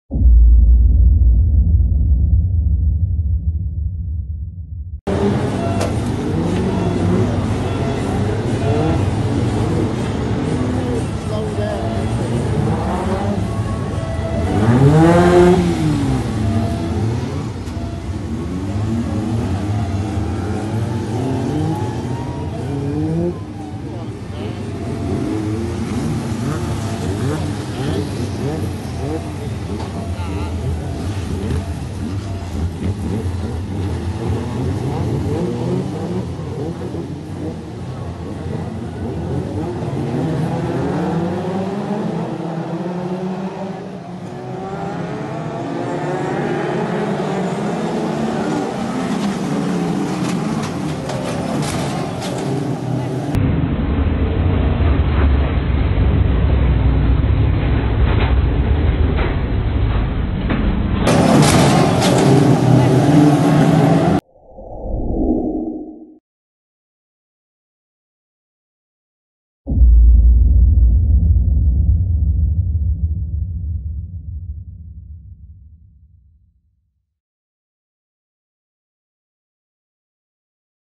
Brisca F2 Skegness Raceway #86 Crash!
Credit to Eastcoast  stockcars photography for the photos

Not my footage! Source unknown.